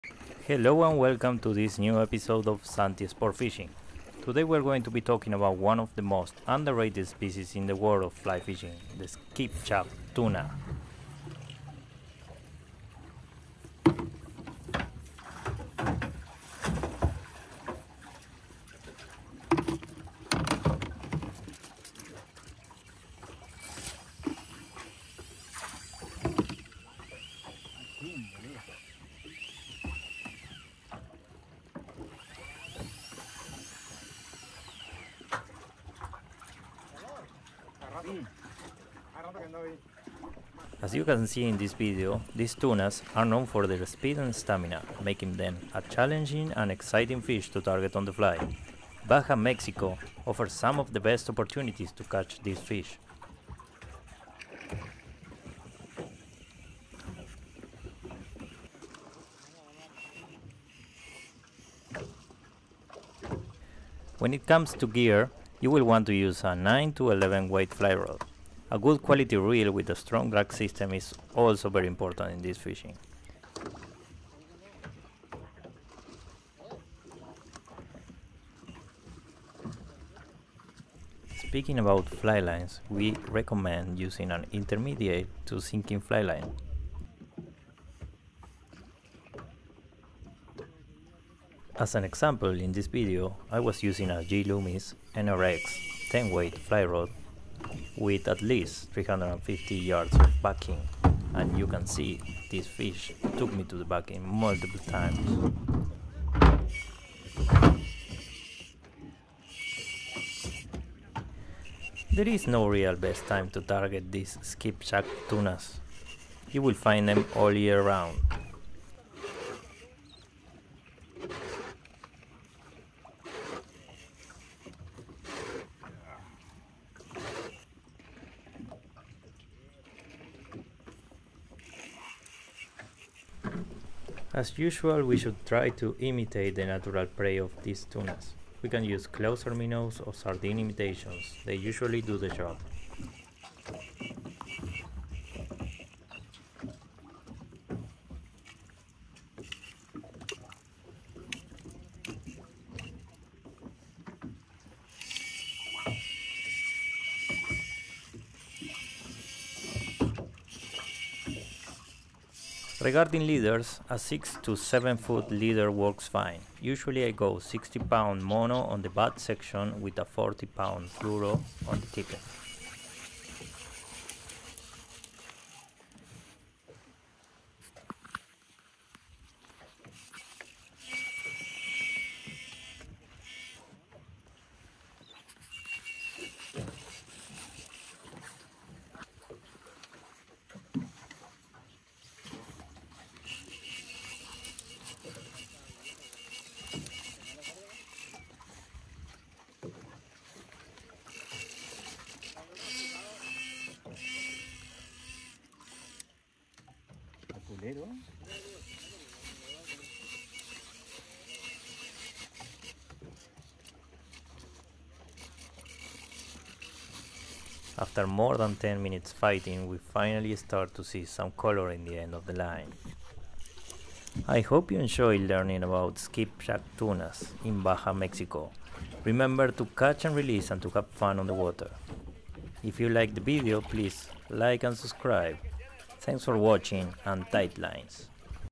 Skipjack Tuna on the fly - Baja Mexico!
Join us on an unforgettable fishing adventure in Baja Mexico as we target the underrated Skipjack Tuna on the fly. Watch as we put our skills to the test against these powerful and fish. With breathtaking scenery and non-stop action, Baja is an incredible place to plan your next fishing vacation.
You will learn everything you ever need to target these fish with a fly rod.

Viaja conmigo en una aventura de pesca nueva einolvidable en Baja, México mientras pescamos al muy subestimado Skipjack Tuna con mosca. Mira cómo ponemos a prueba nuestras habilidades contra estos atunes chicos pero poderosos. Con paisajes impresionantes y acción continua, Baja es un lugar increíble para planificar tus próximas vacaciones de pesca. ¡No te pierdas la diversión, presiona el botón de suscripción y unite al viaje!
Vas a aprender todo lo que se necesita para pescar estos peces con una caña de mosca.